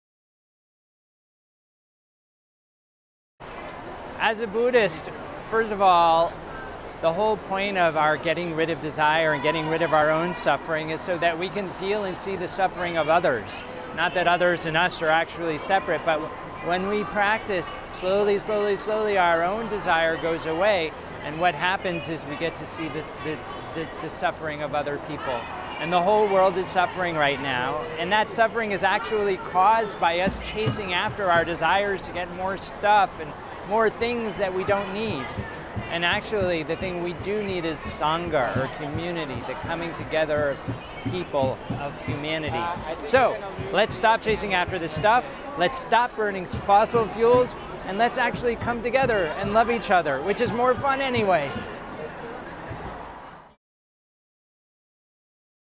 Colin Beavan aka 'No Impact Man'
Colin Beavan speaks on the importance of community at the Youth Climate Strike in New York City.